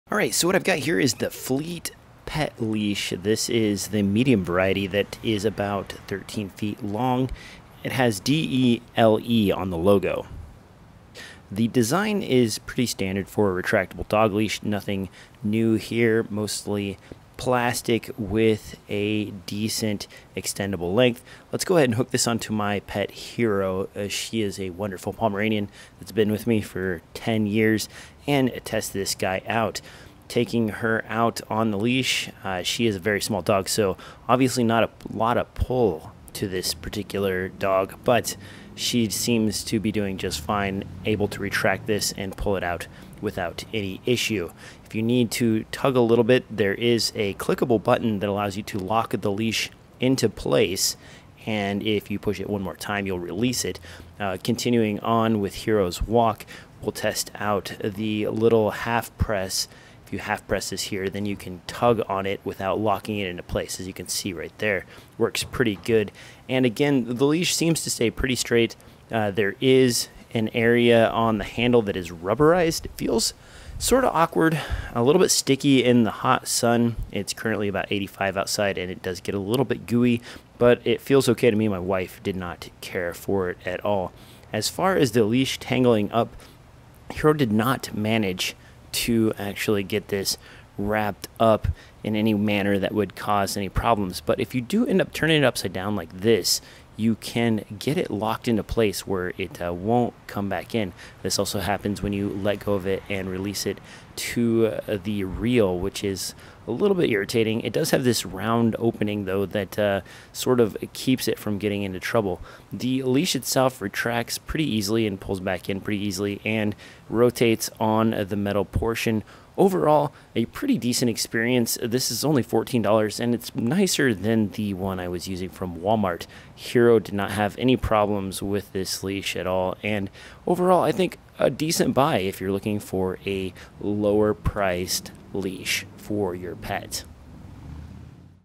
Flytt Retractable Dog Leash review
At $14, it's a dog leash that does a pretty decent job for the price. Great for small and large dogs alike. Rubber grip takes some getting used to, but overall a very nice leash.